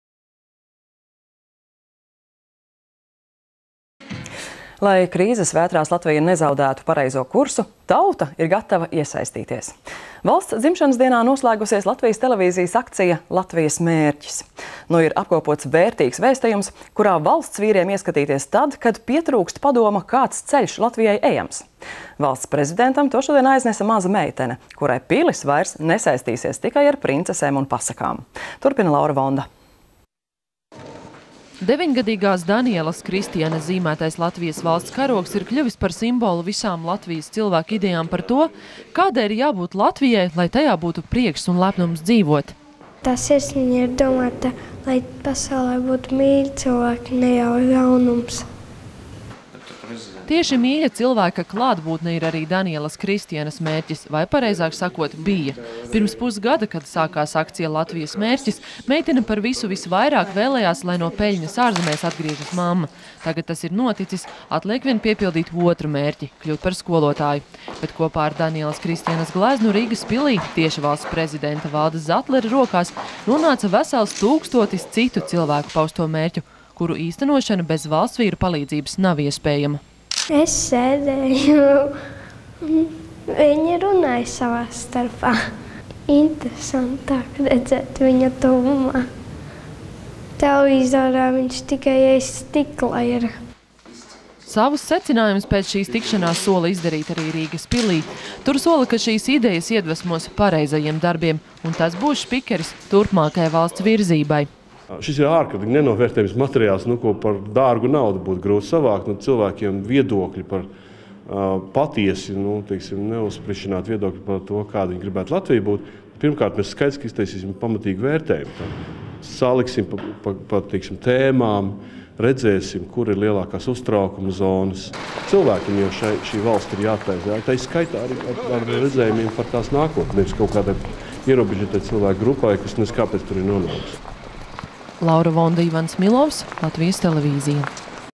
Noslēdzas akcija «Latvijas mērkis»
Lai krīzes vētrās Latvija nezaudētu pareizo kursu, tauta ir gatva iesaistīties!  Valsts dzimšanas dienā noslēgusies LTV akcija «Latvijas mērķis». Tās gaistā apkopts vērtīgs vēstījums, kurā ieskatīties valstsvīriem tad, kad pietrūkst padoma, kāds ceļš Latvijai tālāk ejams. To šodien valsts prezidentam aiznesa maza mitene, kurai nu pils vairs nesaistīsie stikai ar princesēm un psakām.